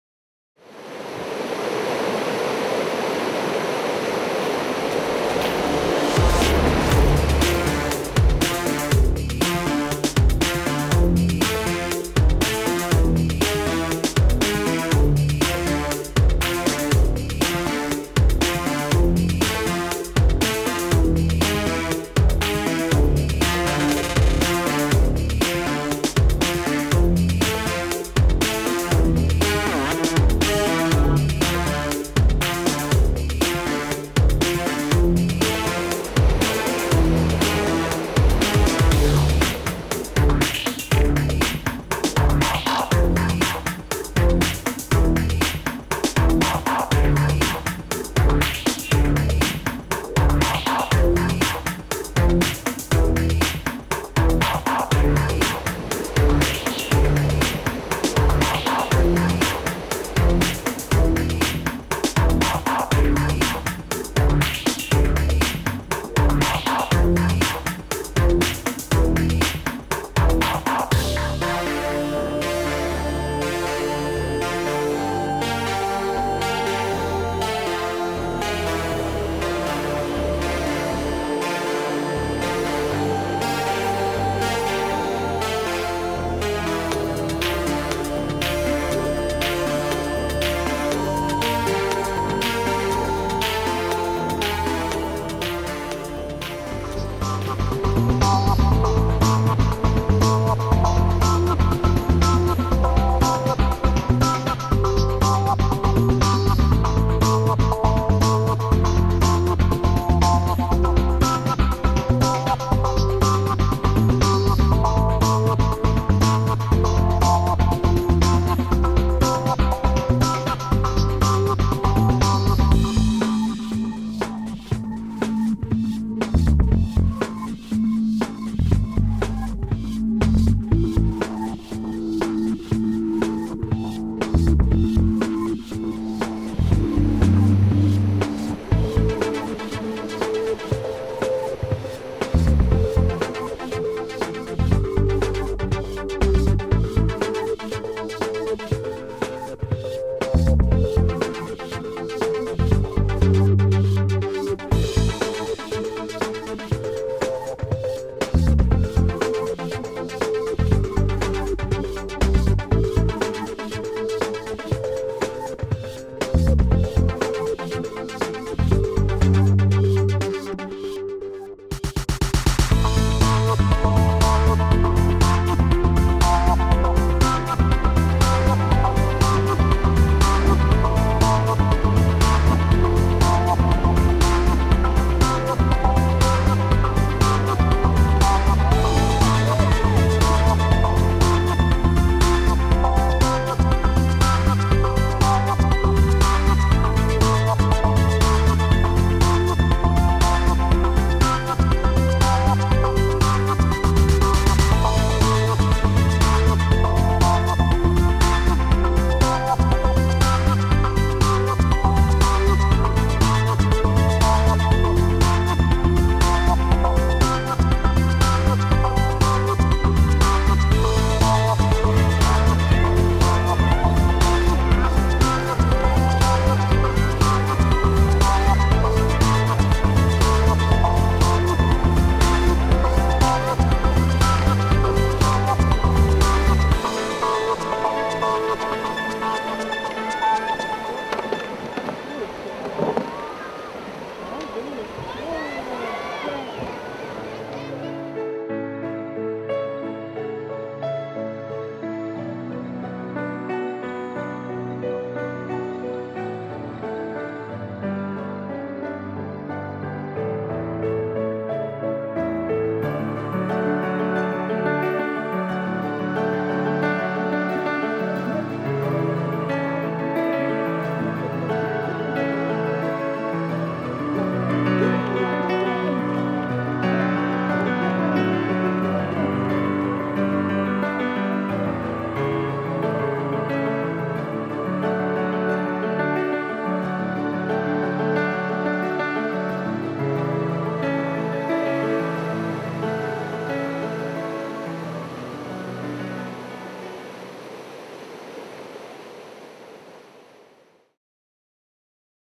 Die Berg
The 240 km Berg River Canoe Marathon from Paarl to Velddrif on the South African West Coast in 5 minutes. A music montage.